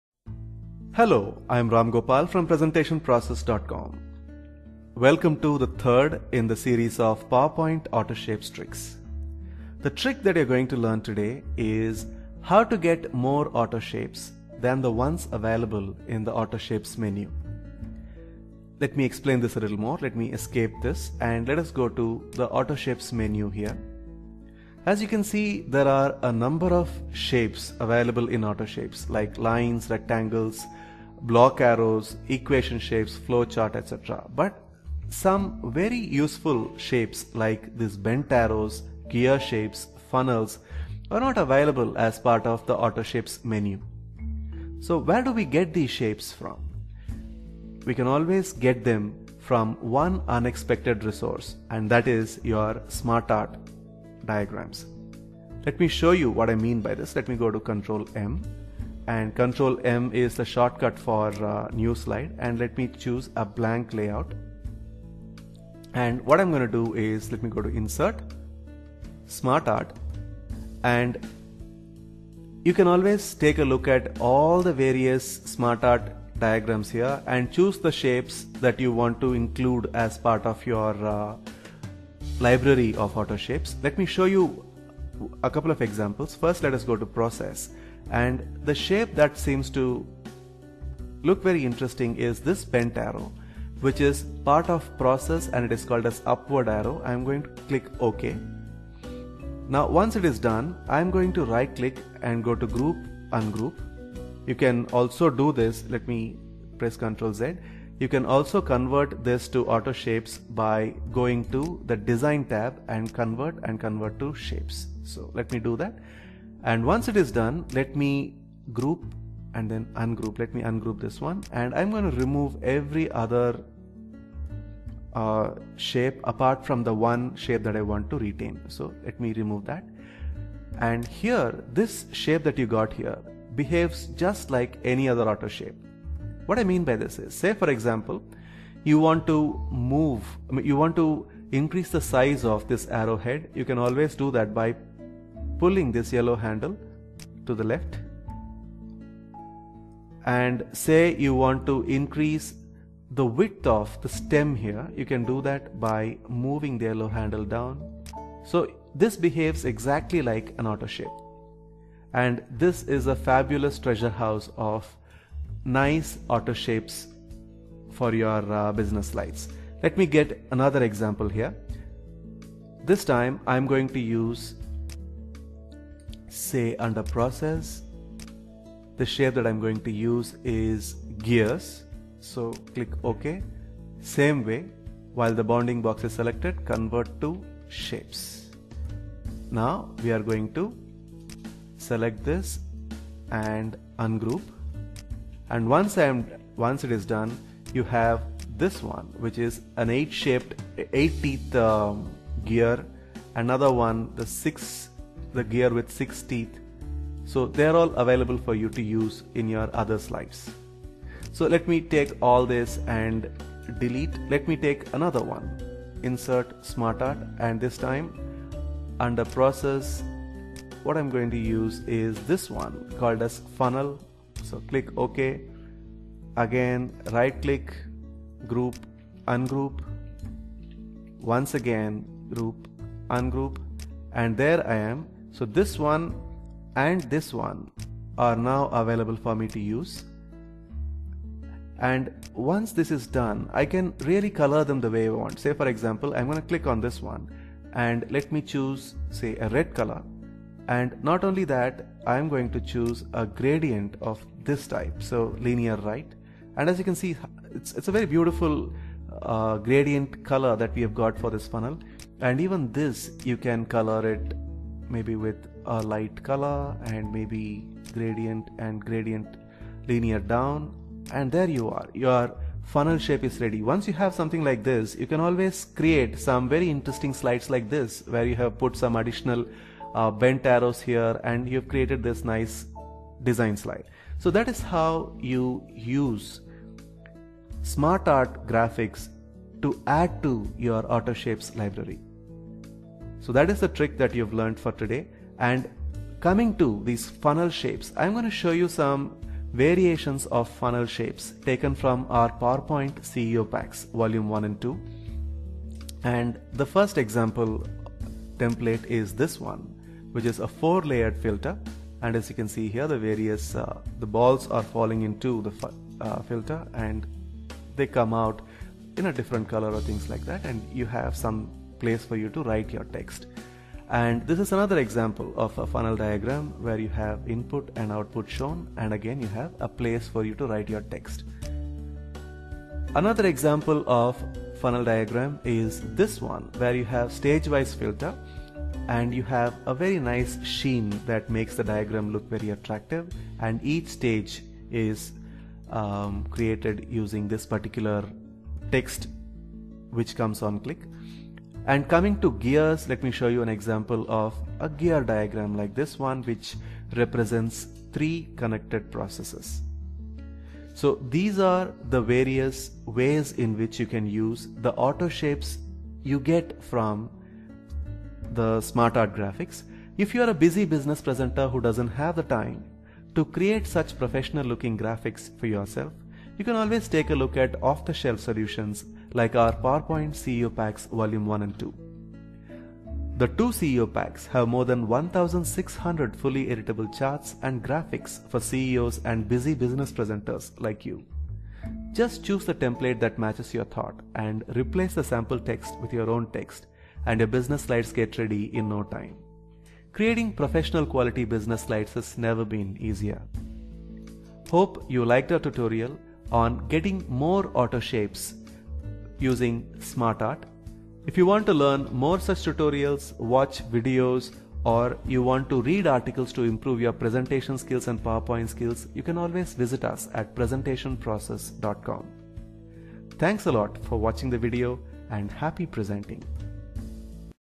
PowerPoint Autoshapes Trick 3 : PowerPoint Effects Tutorials
Learn to find hidden new autoshapes using Smartart feature in PowerPoint. This tutorial is part of the Autoshapes tricks series.

Browse the PowerPoint CEO Pack Bundle mentioned in the video